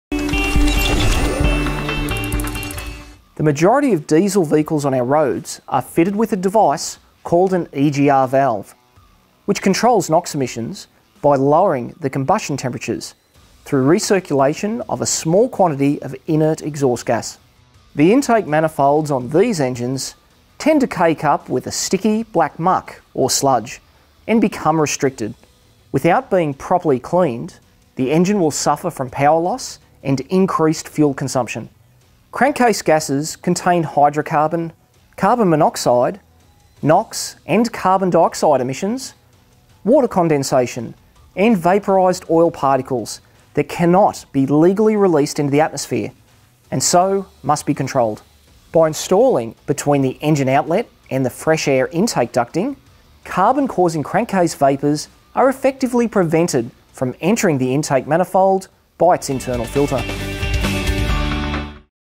Crankcase Ventilation and Diesel Engines (teaser)
The Ryco Filters Crankcase Filtration System module reviews crankcase gasses, how they are produced, and the side effects of EGR and PCV control in both gasoline and diesel engines. By installing a Ryco Crankcase filtration system to affected engines, sludge and carbon build up is all but eliminated. The discussion includes: the operation and function of a PCV system, EGR system requirements, and side effects of emission control and installation and operation of a Ryco Crankcase Filtration System. See the full video in our play list.